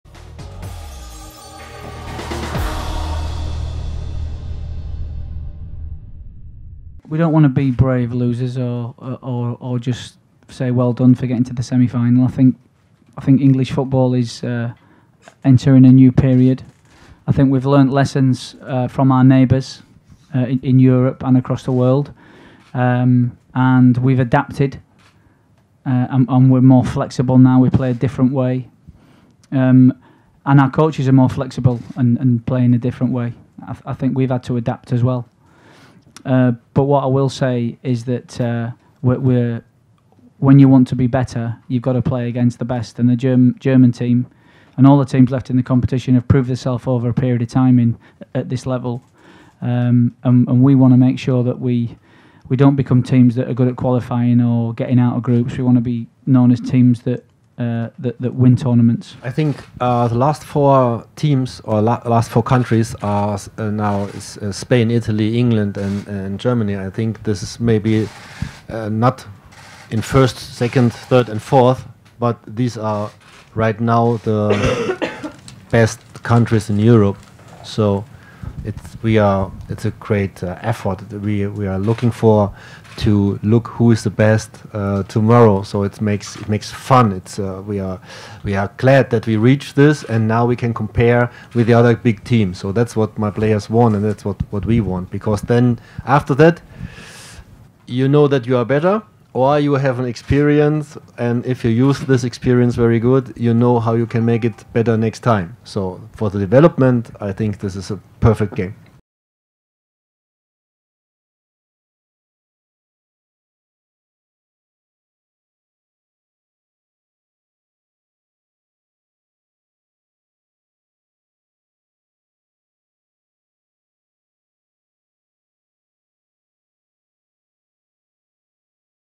England U21 v Germany U21 - Managers Preview The Semi-Final
England U21 manager Aidy Boothroyd and Germany U21 manager Stefan Kuntz preview their Euro semi-final clash in Poland.

Thanks